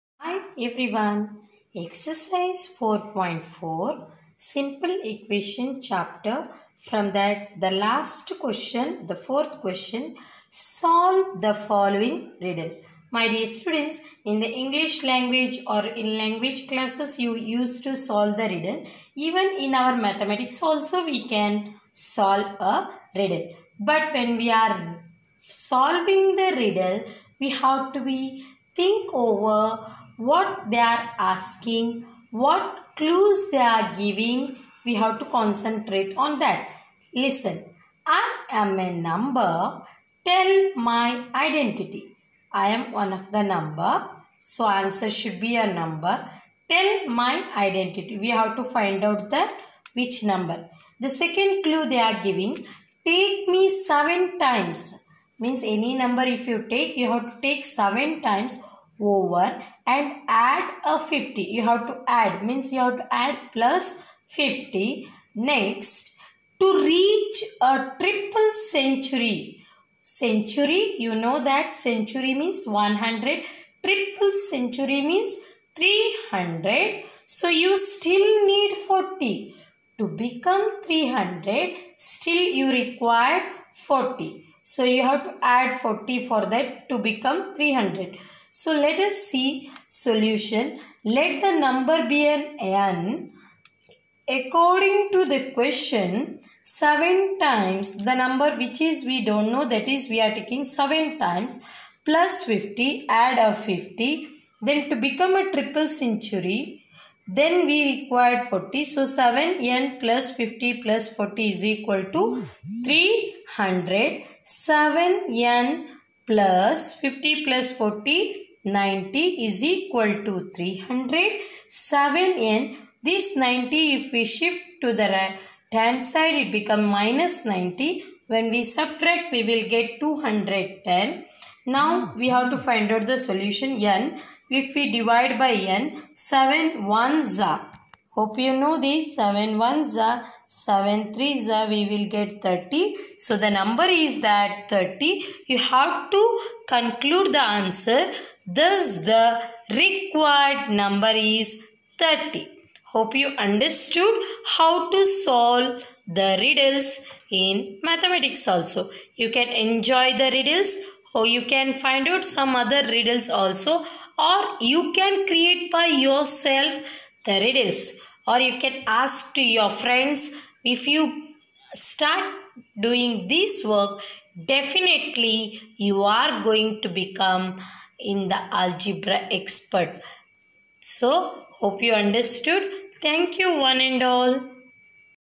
How to solve riddle in Math Algebra/Exercise 4.4 Q. NO. 4
To turn the English into Algebra it helps to:

Read the whole thing first

Do a sketch if possible

Assign letters for the values

Find or work out formulas

You should also write down what is actually being asked for, so you know where you are going and when you have arrived!